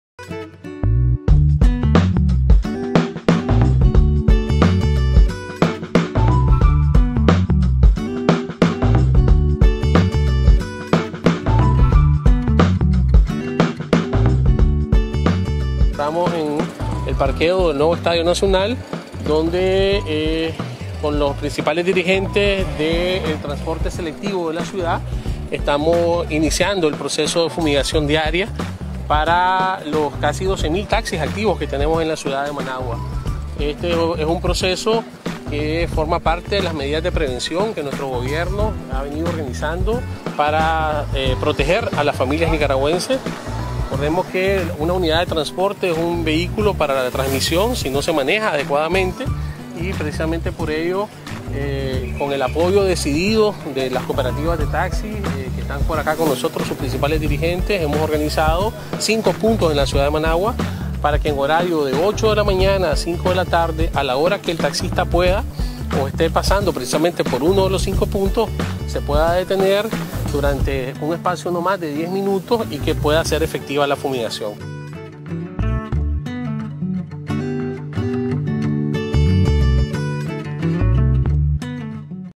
Jornada de Desinfeccion en Taxis de Managua y en el resto del país
Alcaldía de Managua